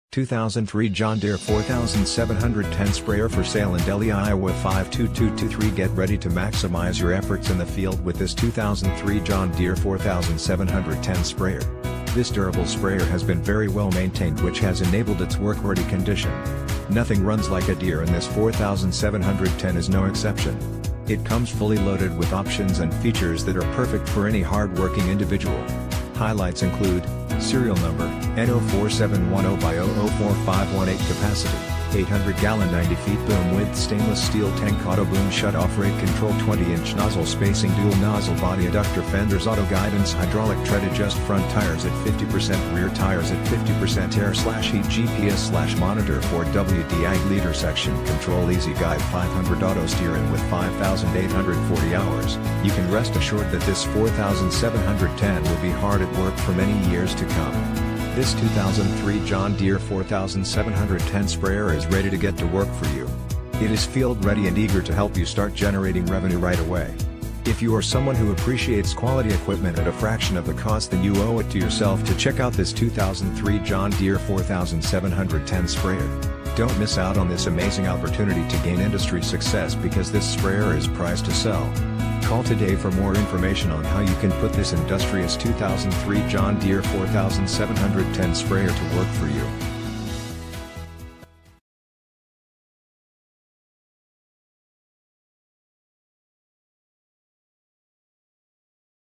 2003 John Deere 4710 JN5564RJ662
2003 John Deere 4710 Sprayer

QuickBye.com

2003 John Deere 4710 Sprayer For Sale in Delhi, Iowa 52223   Get ready to maximize your efforts in the field with this 2003 John Deere 4710 Sprayer . This durable sprayer has been very well maintained which has enabled its work ready condition. Nothing runs like a Deere and this 4710 is no exception. It comes fully loaded with options and features that are perfect for any hard-working individual.   Highlights Include:  Serial Number: N04710X004518 Capacity: 800 Gallon 90 Feet Boom Width Stainless Steel Tank Auto Boom Shut Off Rate Control 20 Inch Nozzle Spacing Dual Nozzle Body Eductor Fenders Auto Guidance Hydraulic Tread Adjust Front Tires AT 50 Percent Rear Tires At 50 Percent Air/Heat/GPS/Monitor 4WD Ag Leader Section Control EZ Guide 500 Auto Steer And With 5,840 Hours, You Can Rest Assured That This 4710 Will Be Hard At Work For Many Years To Come!!! This 2003 John Deere 4710 Sprayer is ready to get to work for you. It is field ready and eager to help you start generating revenue right away. If you are someone who appreciates quality equipment at a fraction of the cost than you owe it to yourself to check out this 2003 John Deere 4710 Sprayer . Don't miss out on this amazing opportunity to gain industry success because this sprayer is priced to sell. Call today for more information on how you can put this industrious 2003 John Deere 4710 Sprayer to work for you!!